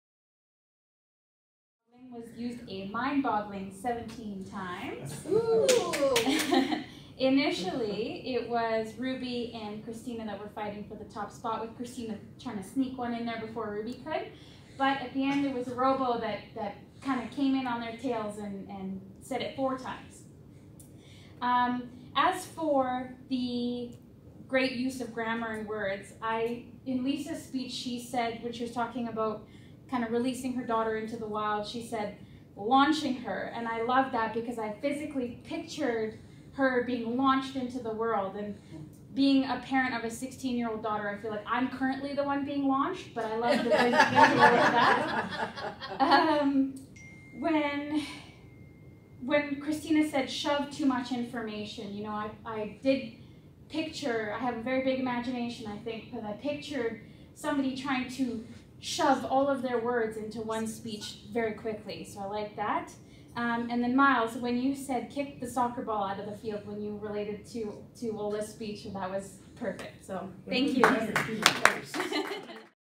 Toastmasters Grammarian Role - Jessi
In her role as the Grammarian, Jessi demonstrates that she listened well during the meeting and noted which phrases the speakers used that really connected with her.